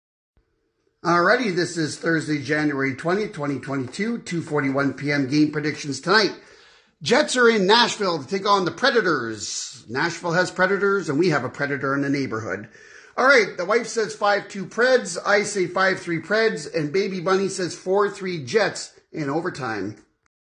UPDATE!!!! Game predictions - Jets @ Predators - 1/20/22
Bunny's wife - 5-2 Predators.
Bunny - 5-3 Preds.
Baby Bunny - 4-3 Jets in overtime.

LOL The wife got this correct! Preds won 5-2.